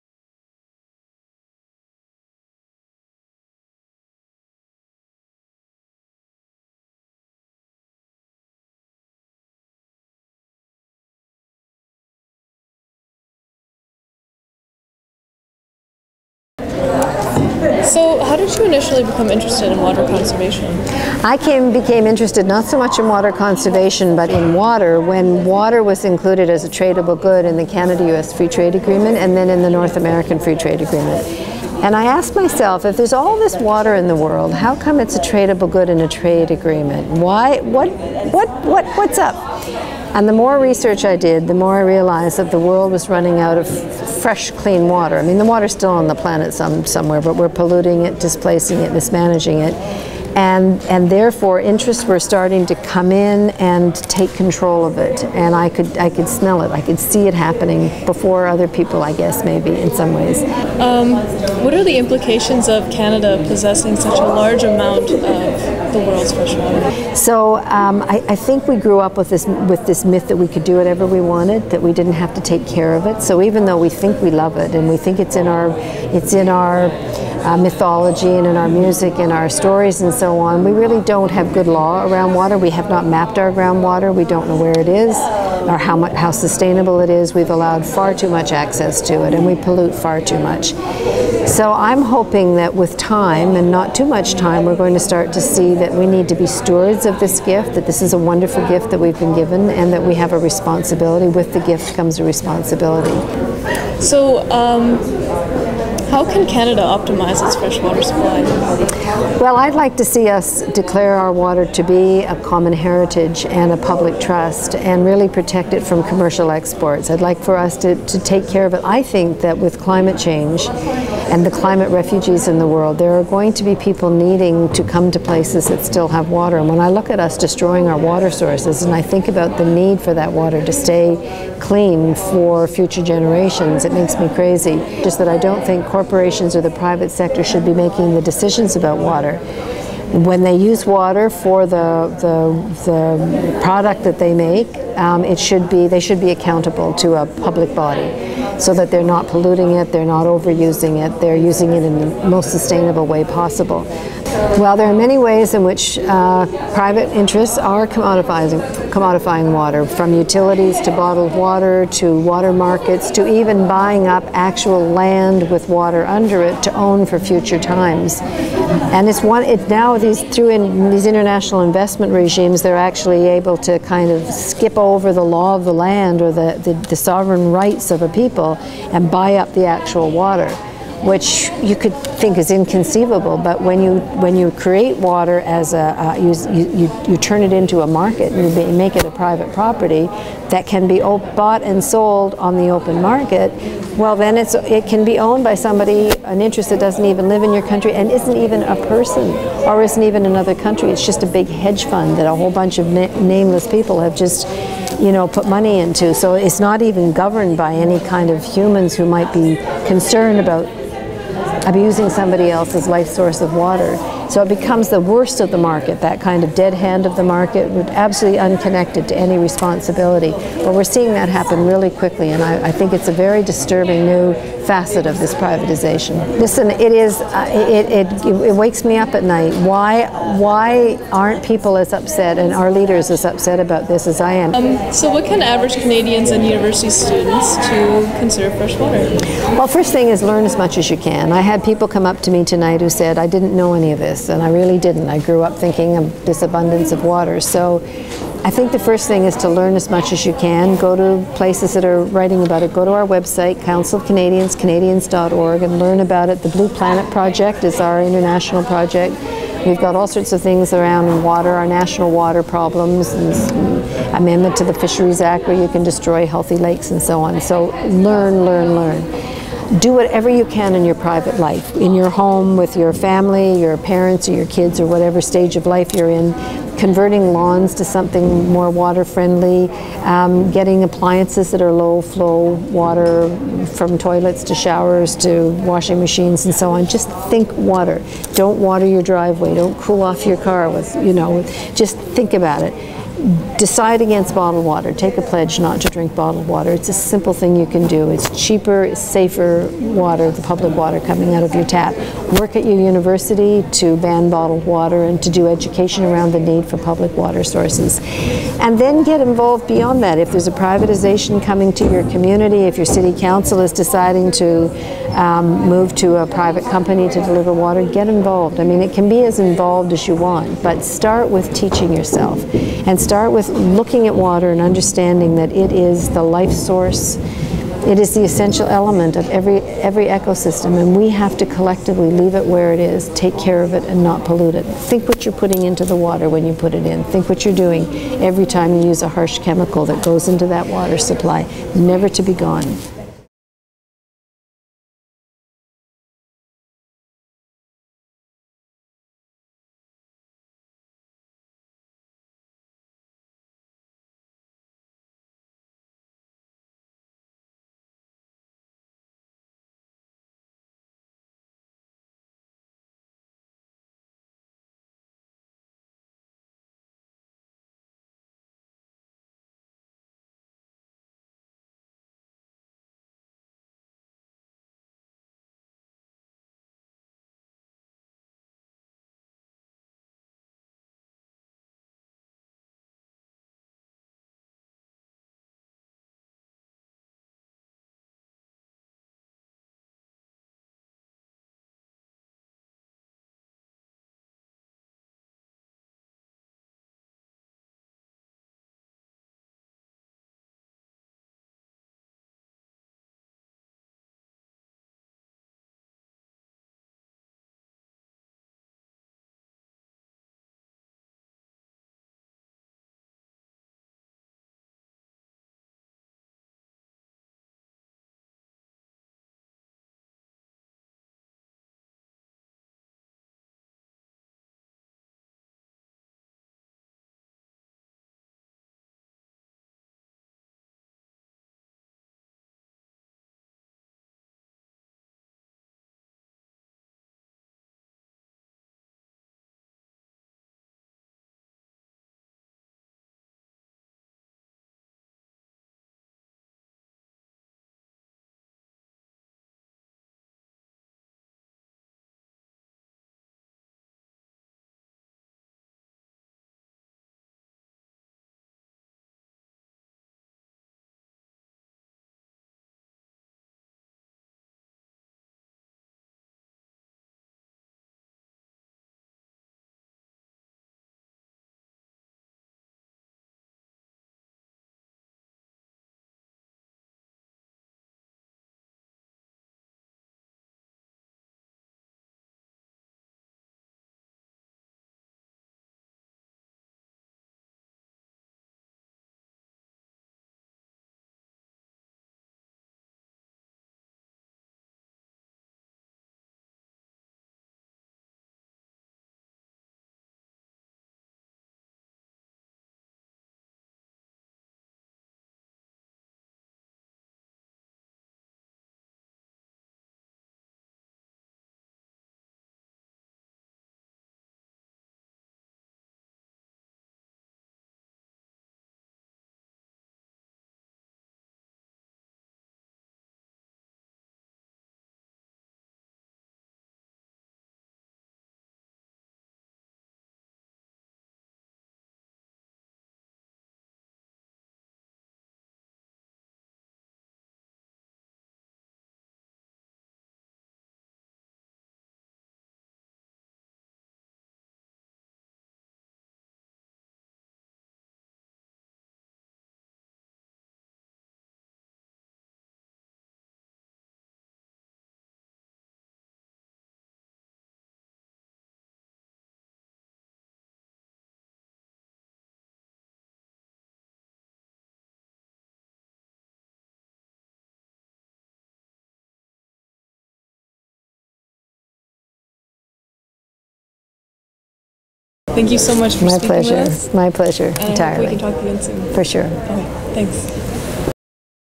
Speaking with Maude Barlow, an environmental activist
U of T's Public Water Initiative hosted a recent lecture about water conservation, and UTTV had the opportunity to interview the keynote speaker about the subject and what we, university students, can do to help the environment!